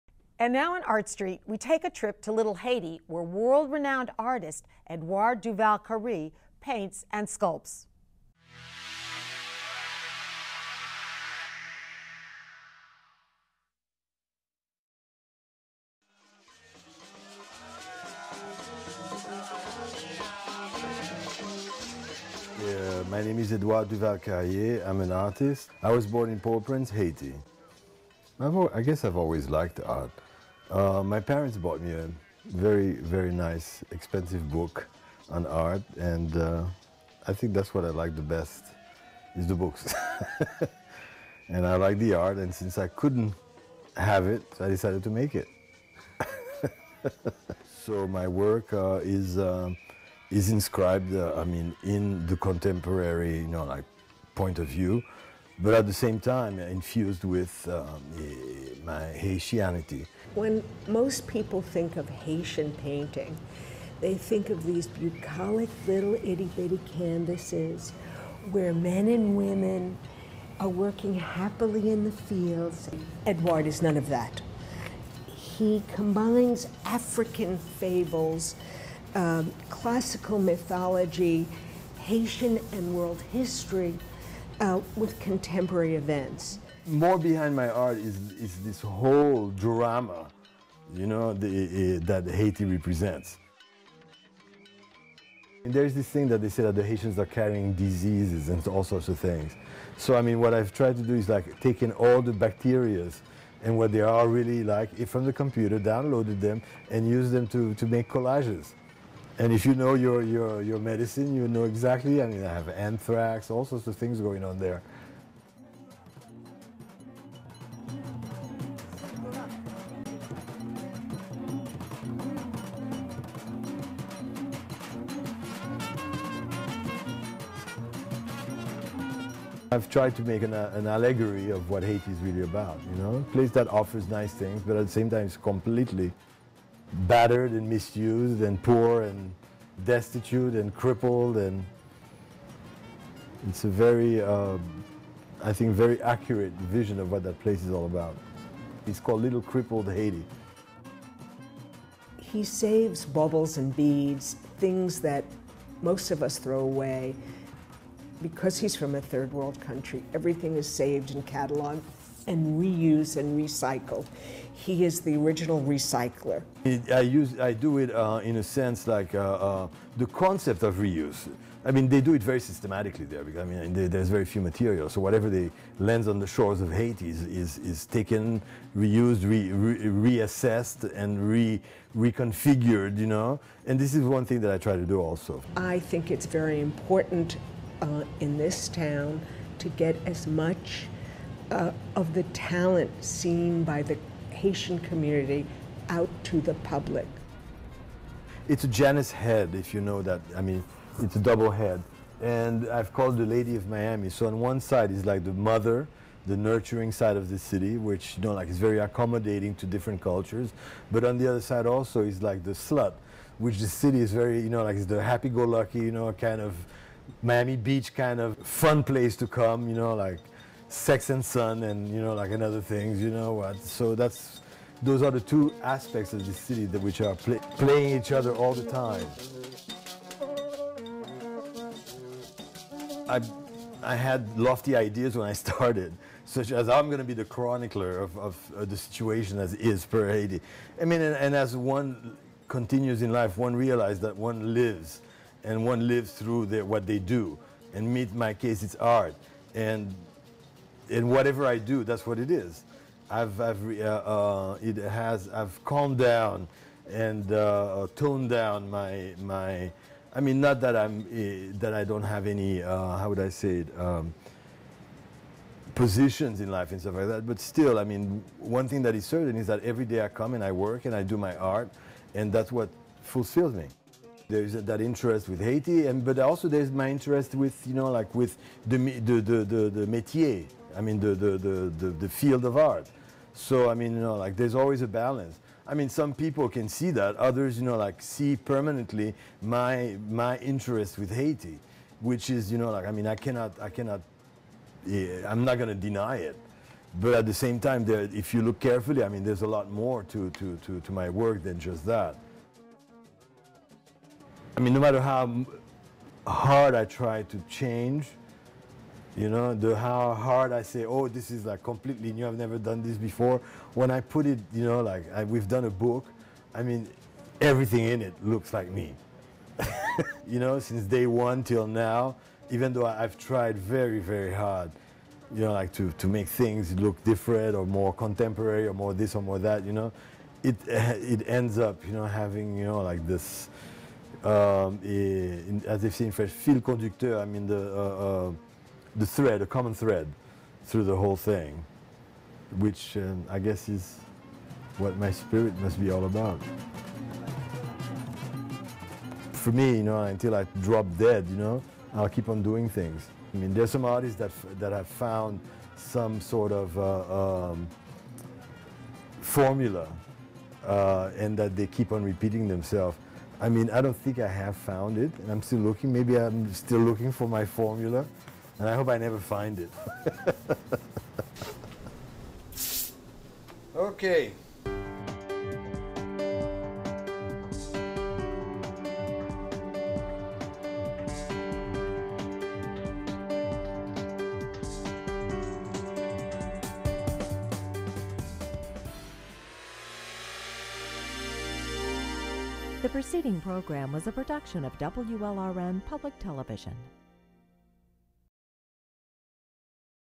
Haitian Artist Edouard Duval-Carrie-Artstreet Miami
Learn the story behind Haitian Artist Edouard Duval-Carrie on this episode.